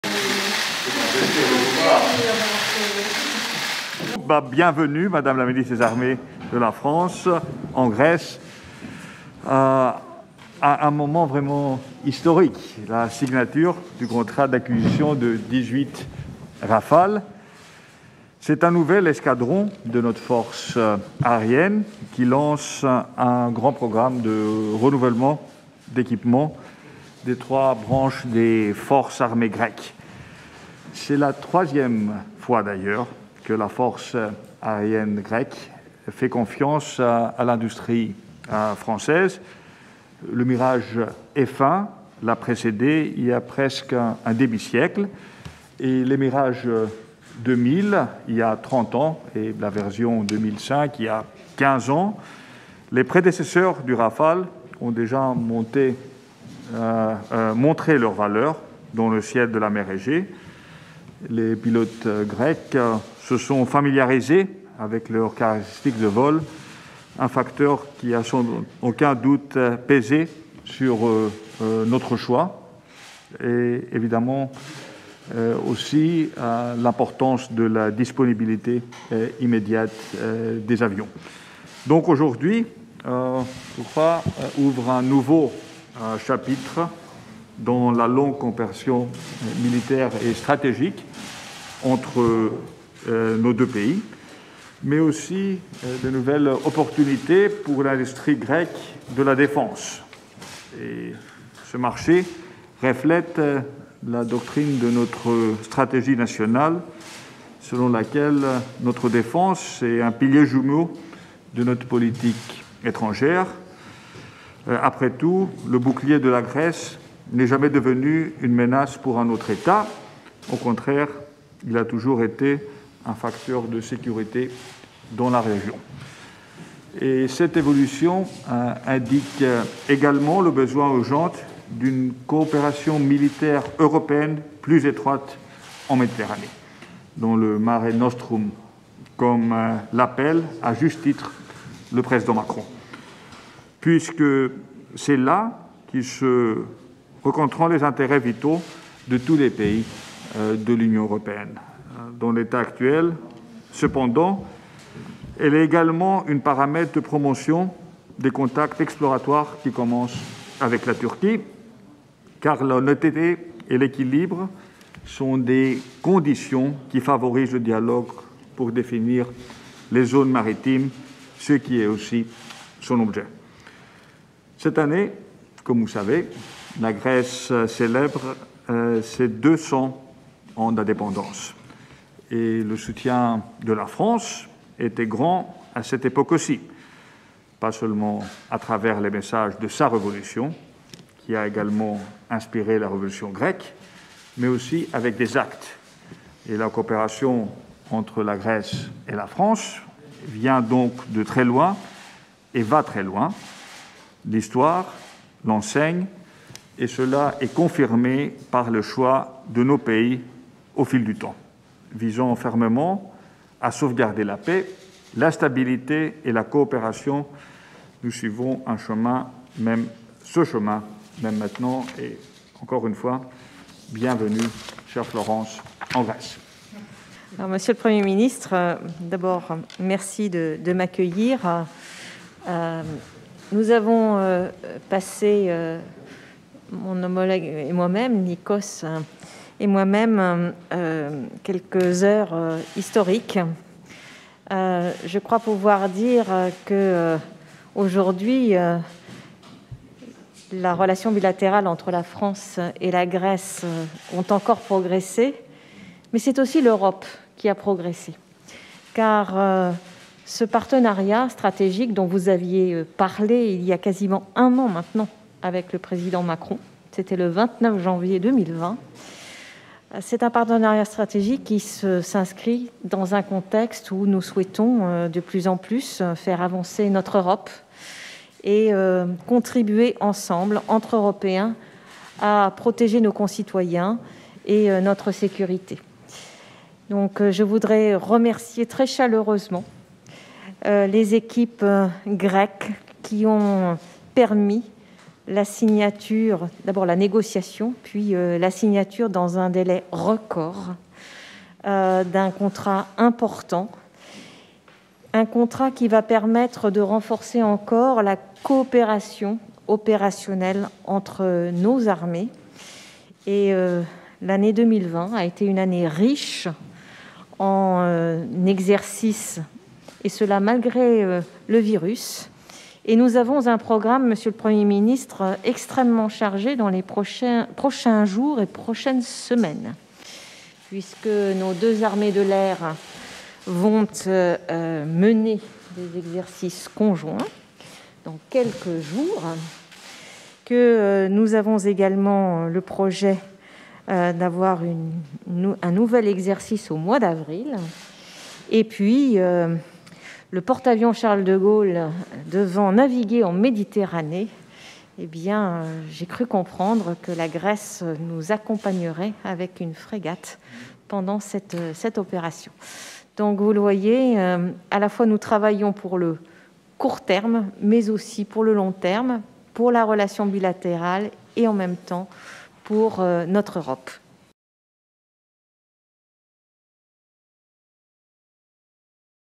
Δηλώσεις του Πρωθυπουργού Κυριάκου Μητσοτάκη με την Υπουργό Άμυνας της Γαλλίας Florence Parly
Δηλώσεις κατά τη συνάντηση του Πρωθυπουργού Κυριάκου Μητσοτάκη με την Υπουργό Άμυνας της Γαλλίας Florence Parly, στο Μέγαρο Μαξίμου